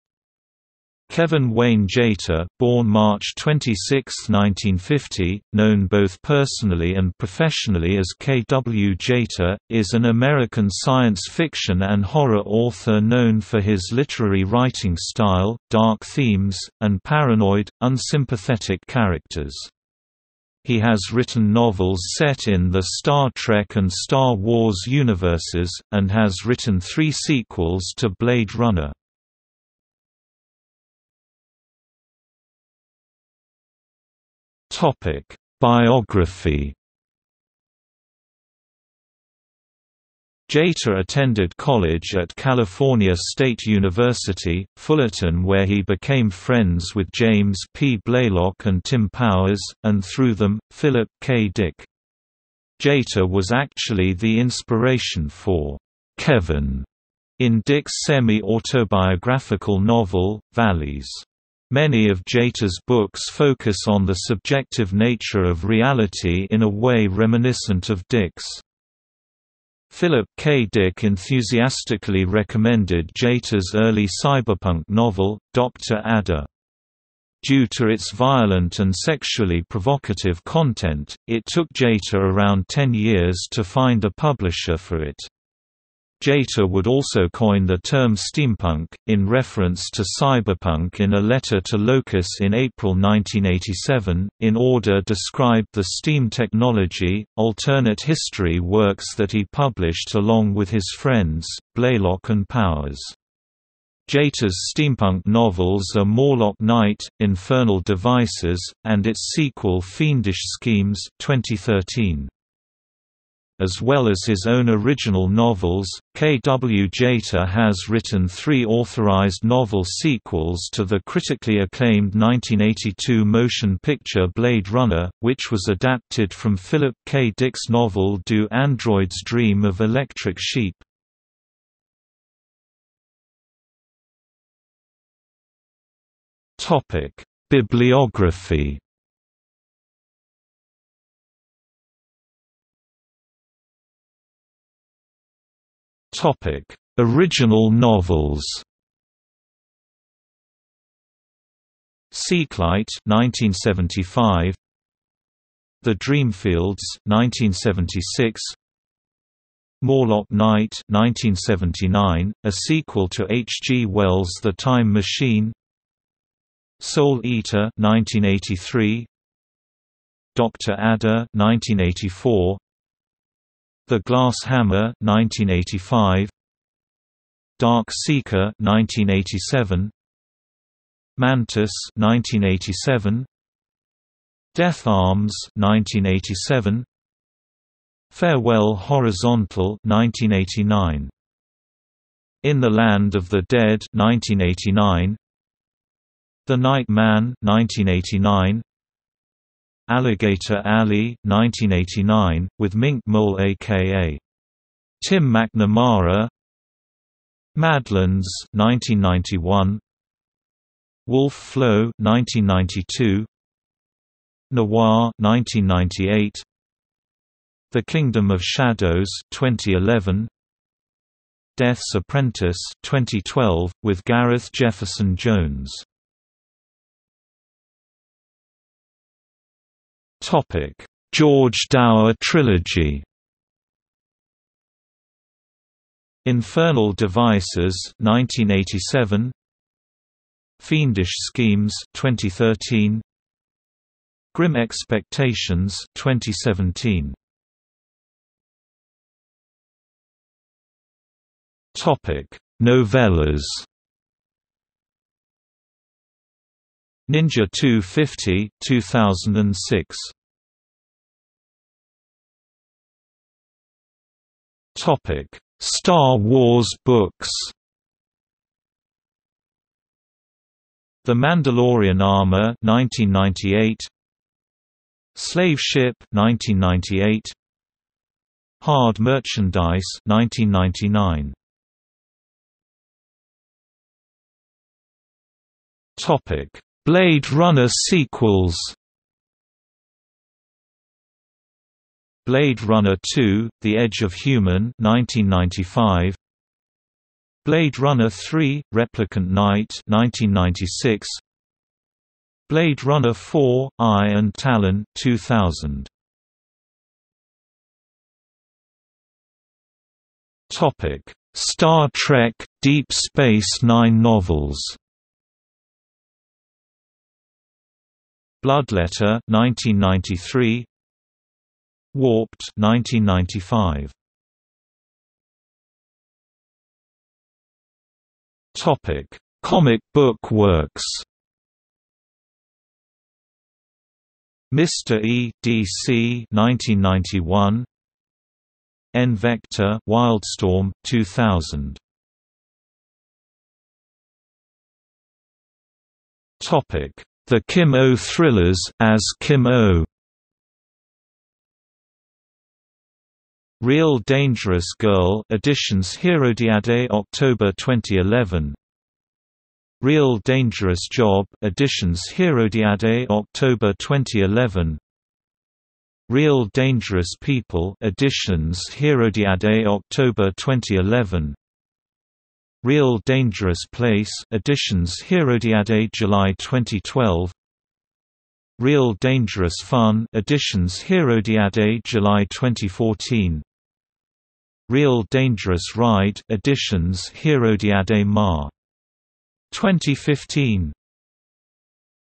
K. W. Jeter | Wikipedia audio article
00:00:31 1 Biography
00:02:12 2 Bibliography
00:02:22 2.1 Original novels
00:03:47 2.1.1 George Dower trilogy
00:04:08 2.2 Novellas
00:04:21 2.3 iStar Wars/i books
00:04:42 2.4 iBlade Runner/i sequels
00:05:07 2.5 iStar Trek: Deep Space Nine/i novels
00:05:25 2.6 Comic book works
00:05:43 2.7 The Kim Oh Thrillers (as Kim Oh)

- Socrates

SUMMARY
Kevin Wayne Jeter (born March 26, 1950), known both personally and professionally as K. W. Jeter, is an American science fiction and horror author known for his literary writing style, dark themes, and paranoid, unsympathetic characters. He has written novels set in the Star Trek and Star Wars universes, and has written three sequels to Blade Runner.